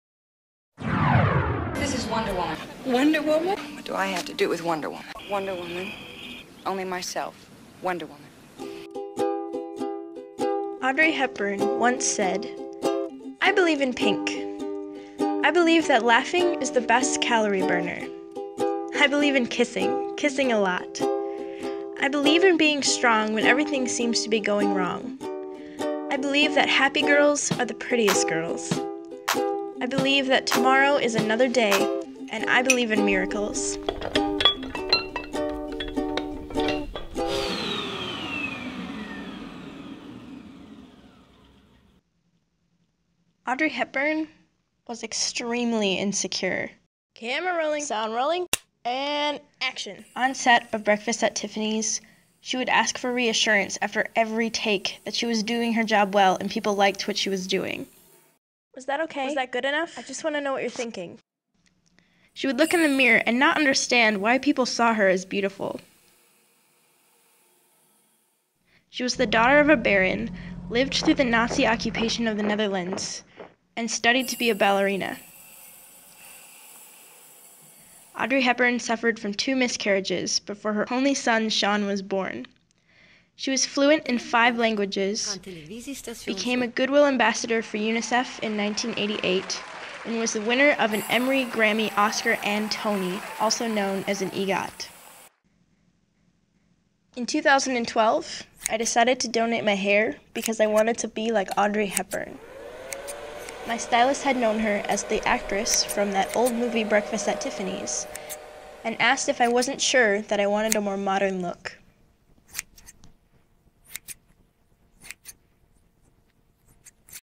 An Audrey Afternoon
This is basically a glorified slideshow for an online class called Film 210. The assignment was to use sound and still images to show something important to you, and I chose Femininity.

It starts off all cute and inspired which I think is how most people see me, and how women are portrayed (even strong iconic women), just like flawless and happy. But then it brings out some truthiness and becomes more complicated, which women are.

I have short hair so one of my biggest 'gender identifiers' is not present, I hesitate to say missing because the whole point of this is that being a woman, to me, is more than just having long hair, wearing dresses and having emotions.

I love Audrey Hepburn because she was such a real person who was beautiful and talented, but also insecure. She user her image to help others and kept being an optimist and an idealist, which I really try to be.